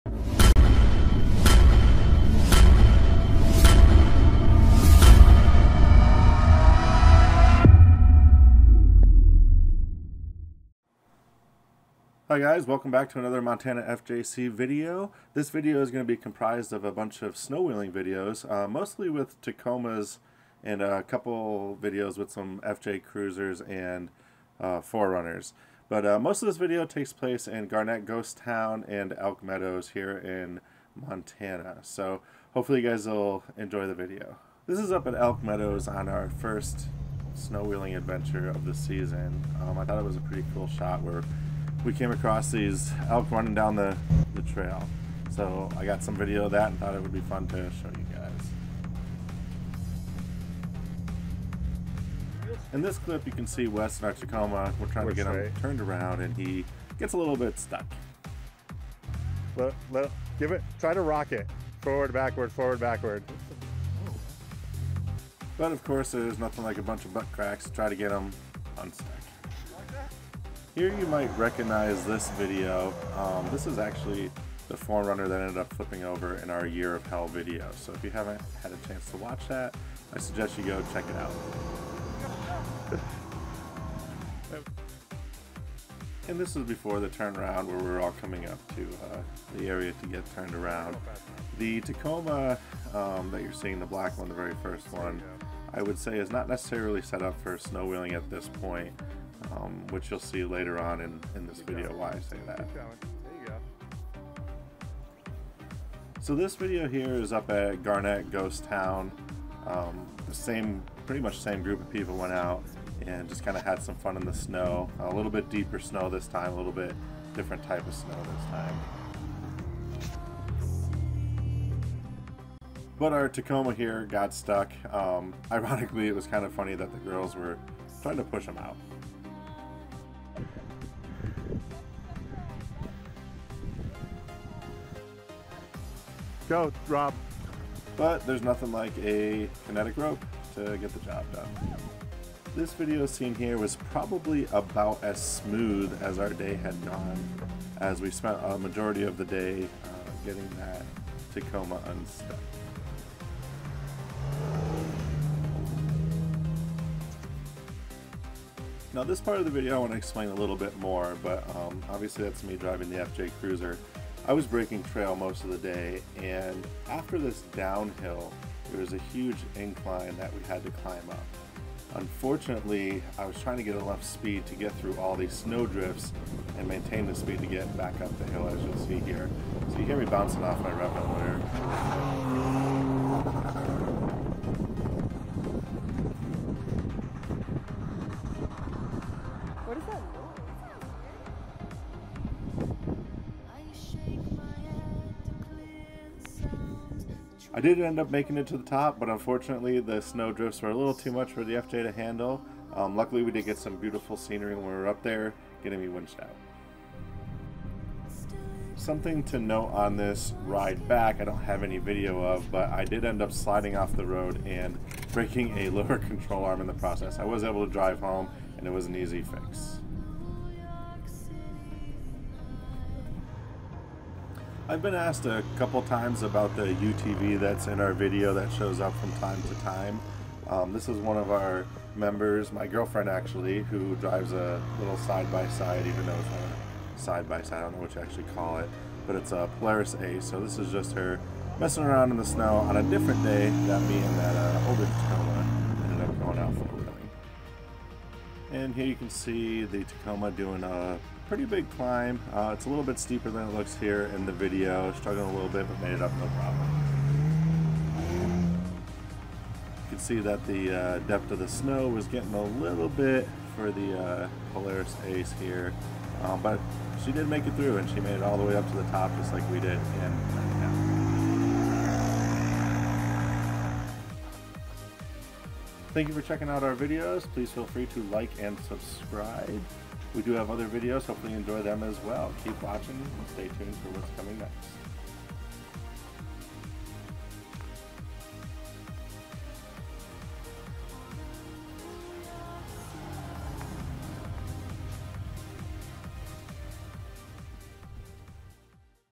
Tacoma Go Snow wheeling /// Polaris Ace /// 4Runner /// FJ Cruiser /// Montana /// Adventure /// 406
Tacoma's take you snowwheeling through Montana back roads on an adventure to Elk medows and garnet ghost town and with them they bring an FJ Cruiser, 4runner and Polaris Ace